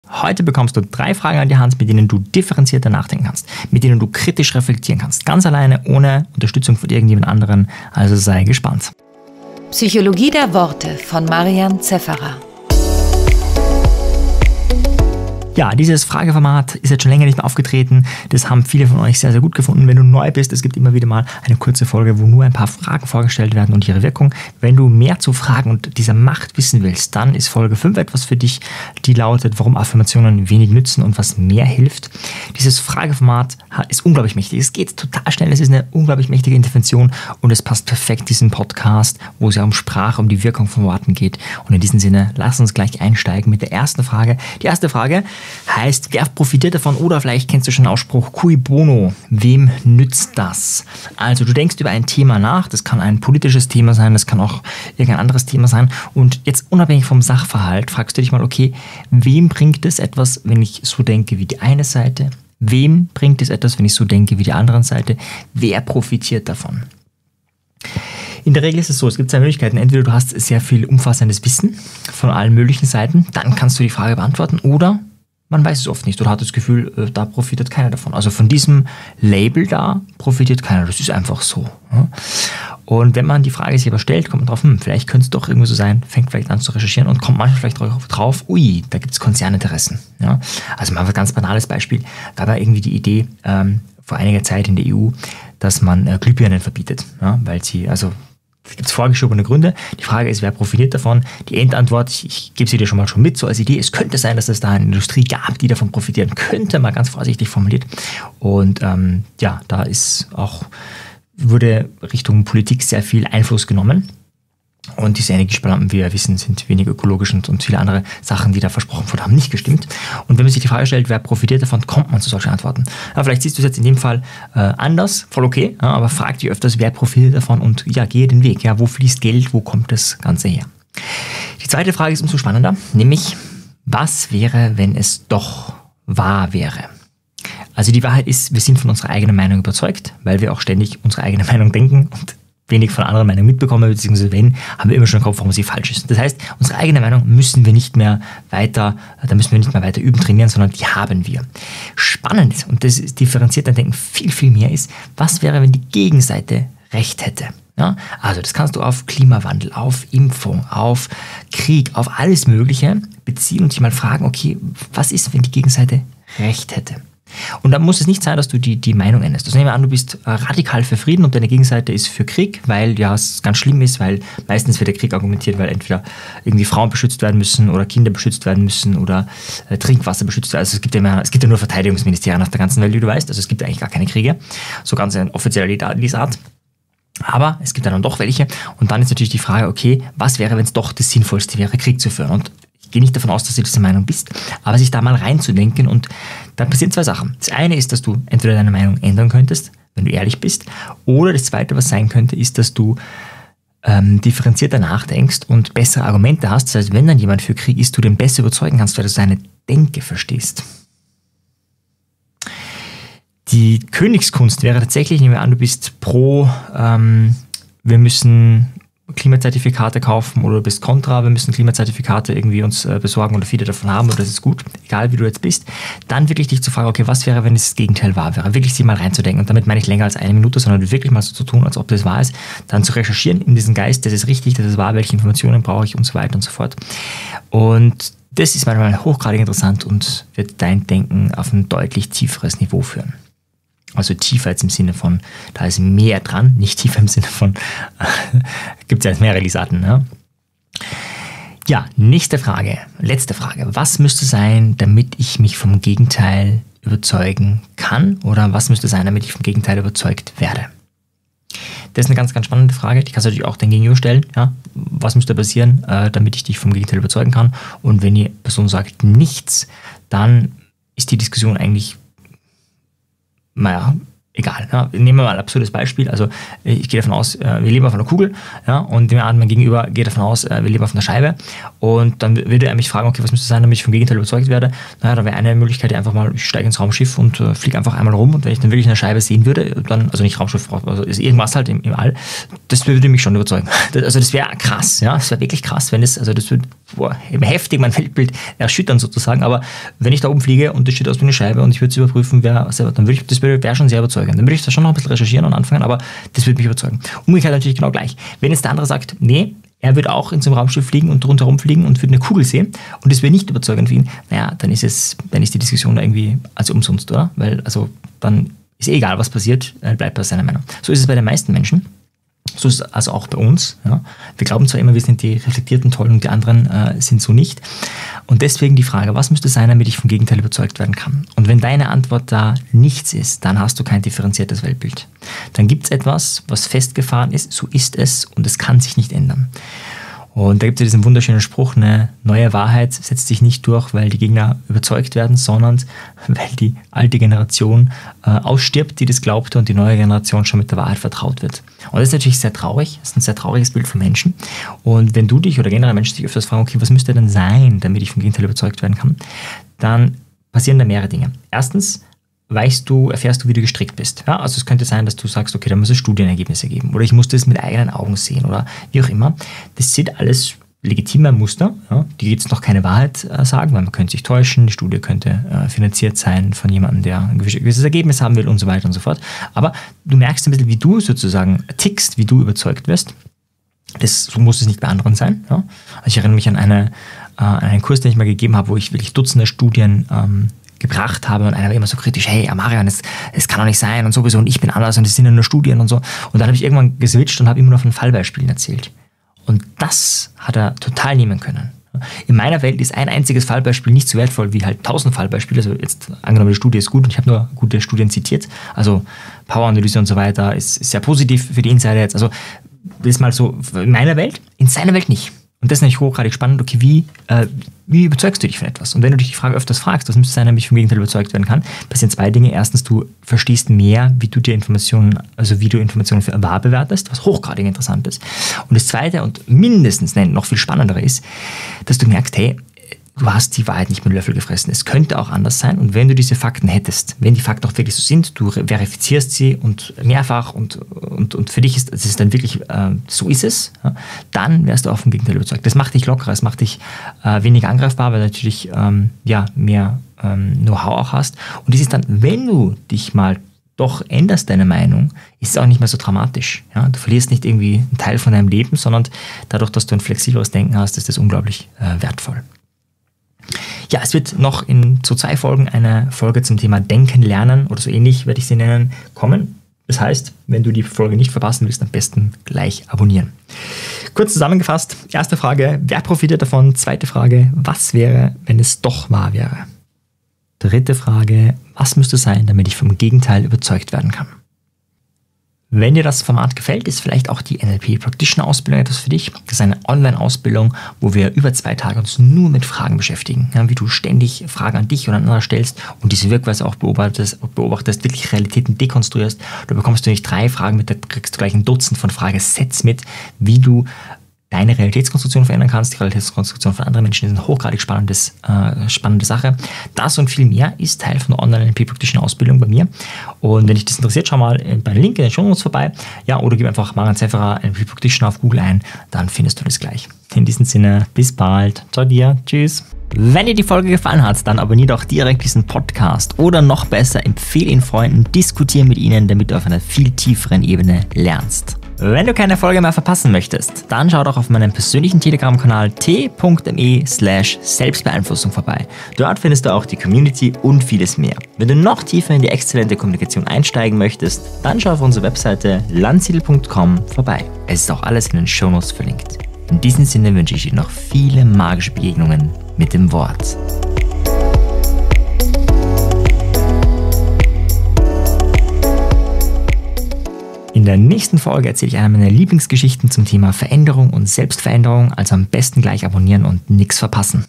Die besten Fragen um differenzierter zu Denken (#046)
Drei Fragen, um differenzierter zu denken. Drei Fragen, um sich selbst infrage zu stellen. Drei Fragen, um wirklich eine eigene Meinung (und keine Anderung) zu erhalten. Das ist Thema dieser Folge, hör unbedingt rein :)